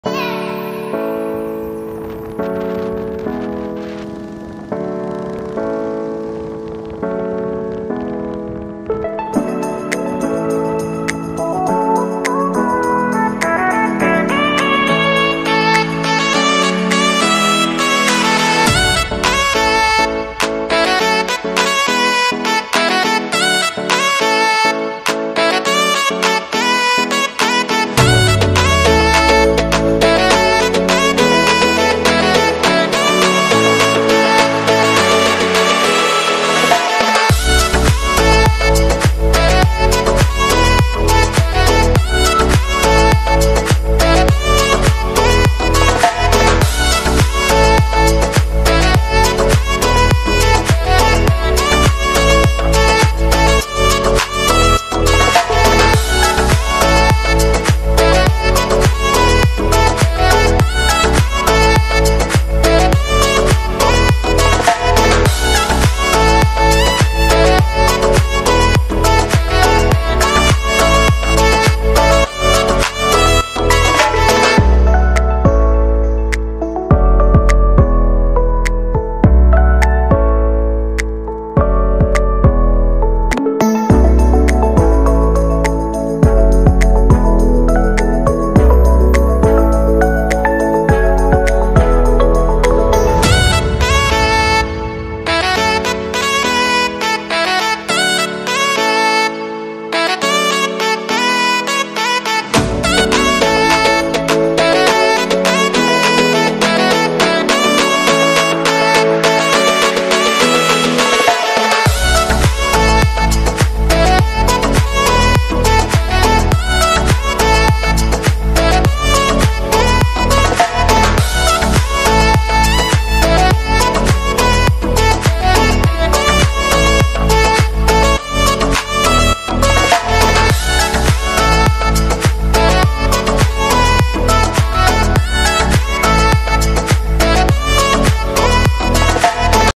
DRT SHORT RIDE WITH WIFEY | GULOD PARADISE | SULIT ANG ENTRANCE FEE
Isa sa pinakamagandang puntahan sa DRT